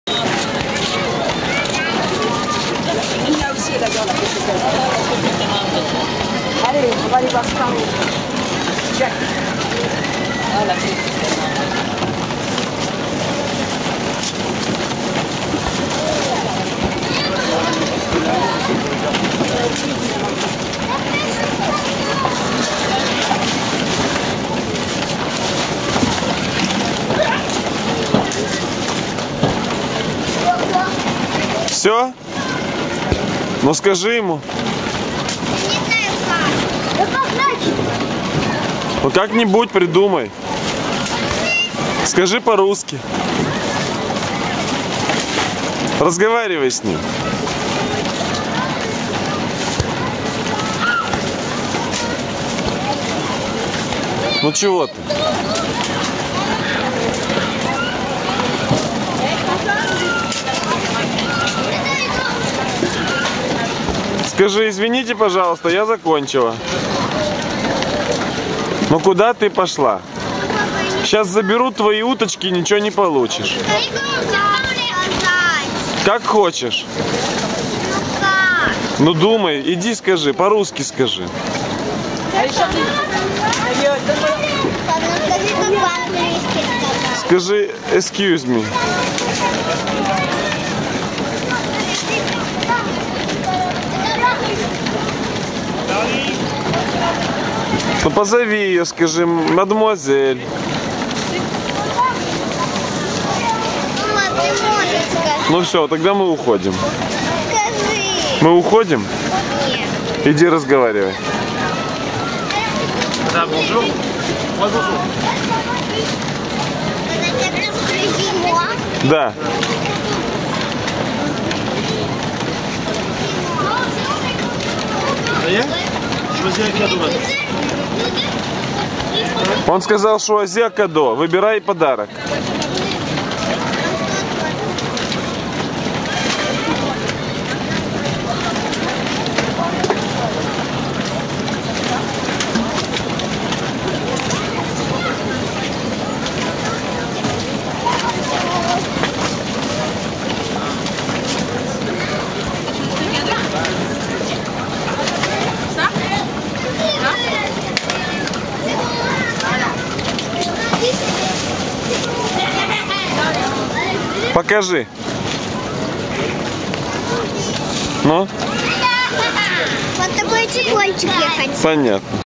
Paris-2010 - Любаша ловит уточек и выбирает приз
13 августа 2010 года. Иль-де-Франс, Виньё-Сюр-Сен, парк развлечений. Детский аттракцион "уточки".